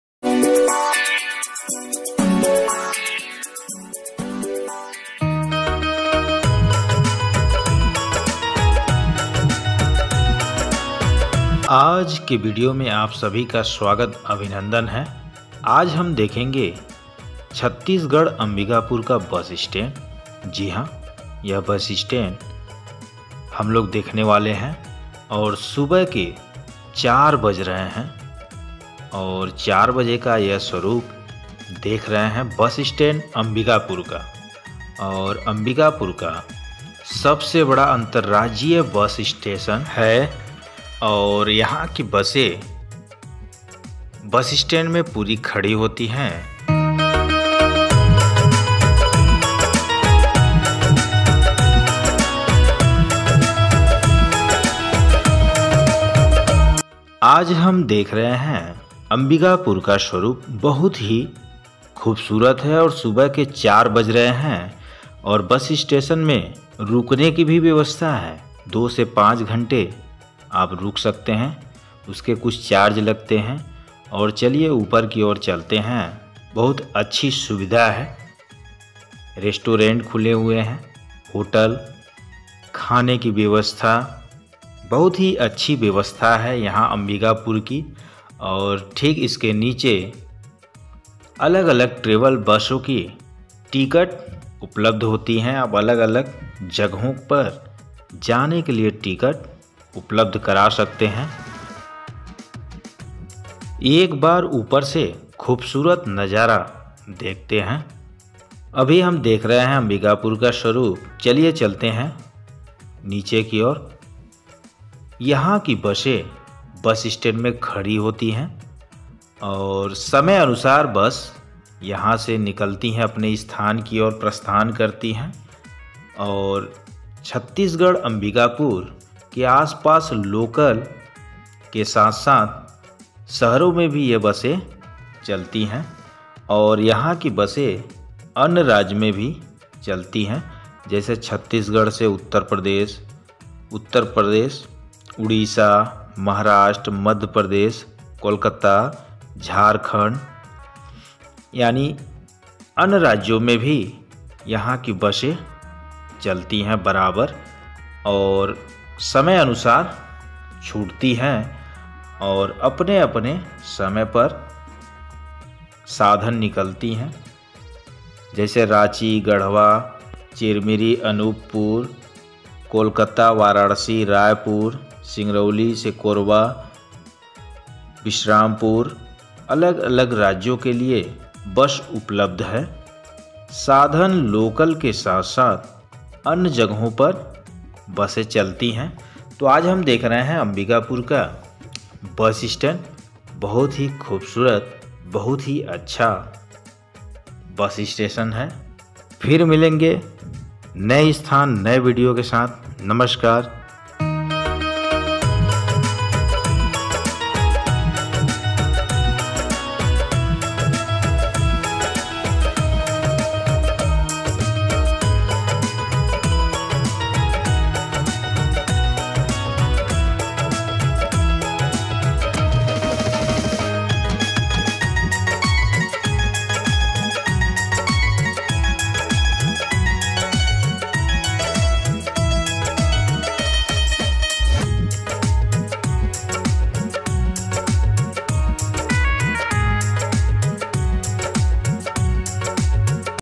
Bus Stand Ambikapur // Chhattisgad Ambikapur Ka Sabse Bada Bus Stand Ambikapur // छत्तीसगढ़ दर्शन ।।
अंबिकापुर बस स्टैंड
छत्तीसगढ़ का बस स्टैंड अंबिकापुर अंतर राज्यिय बस स्टैंड है यहां से पूरी बसे साधन उपलब्ध है हर क्षेत्र में जाने के लिए आज हम देख रहे हैं अंबिकापुर का बस स्टैंड छत्तीसगढ़ ।।